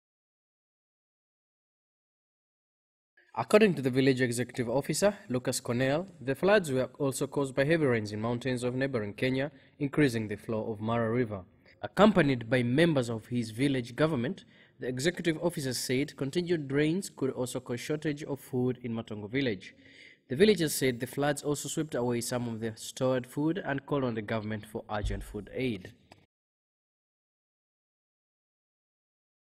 Heavy rains in Tarime district destroy more than ten acres of maize farms.
Heavy rains in Tarime district Mara region have swept away more than ten thousand acres of maize in Matongo village and burst the banks of Mara and Tigete rivers.